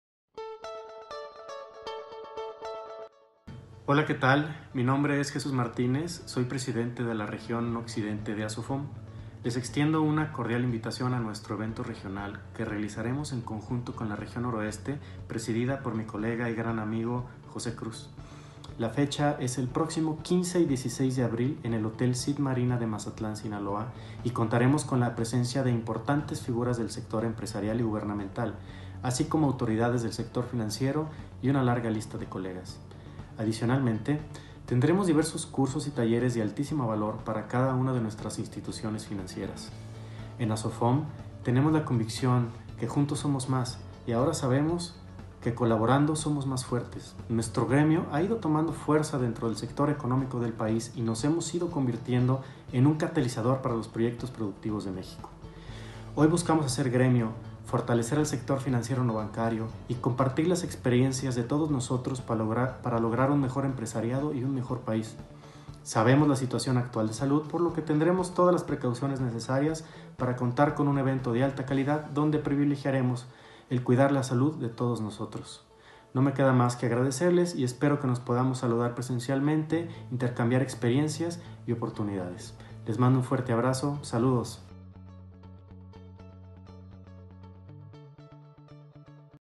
Encuentro Juntos en el Pacífico ASOFOM 2021
Nuestro Presidente Regional de Occidente, Jesús Martínez, te invita a formar parte el próximo 15 y 16 de Abril en el Hotel Cid, Mazatlán, a nuestro evento donde contaremos con ponencias de alto nivel, así como capacitación por parte de especialistas del sector no bancario.